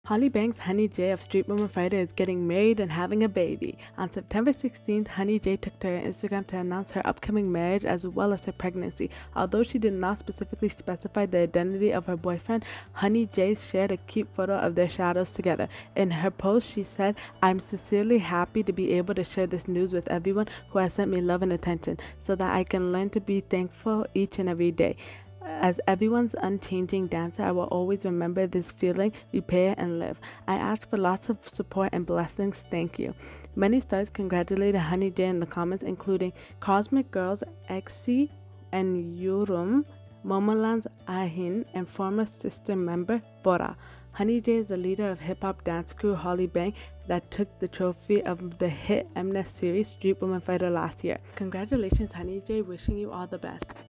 Dancer Honey J Announces Marriage And Pregnancy!!
CONGRATULATIONS HONEY J